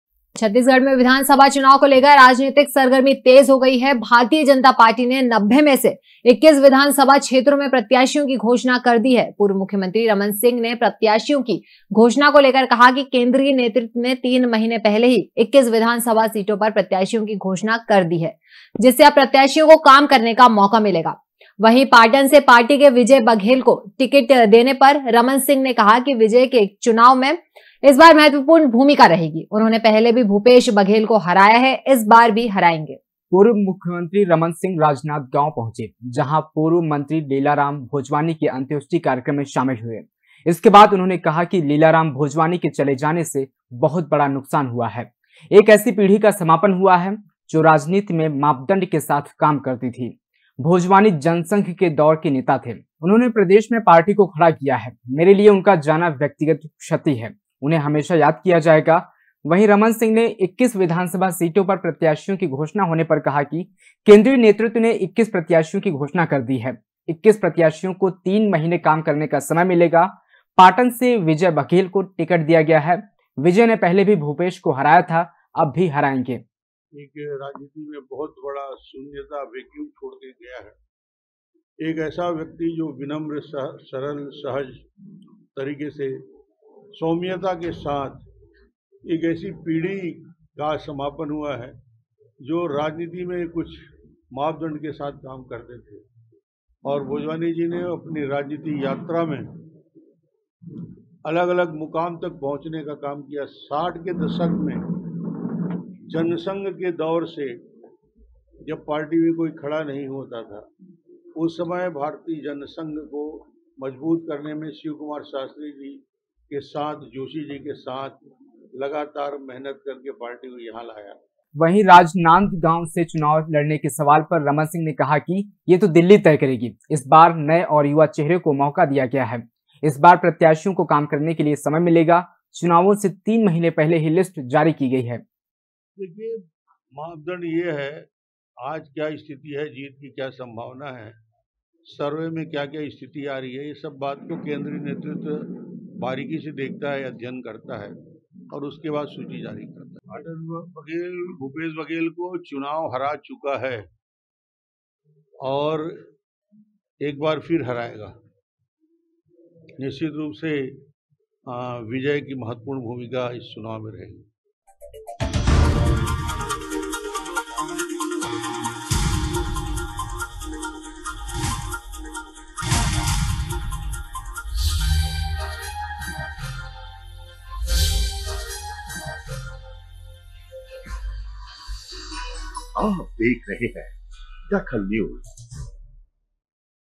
DAKHAL NEWS पूर्व मुख्यमंत्री रमन सिंह राजनांदगांव पहुंचे
पूर्व मुख्यमंत्री रमन सिंह राजनांदगांव पहुंचे
रमन सिंह ने साधा भूपेश बघेल पर निशाना
इस बार चुनाव हारेंगे मुख्यमंत्री भूपेश बघेल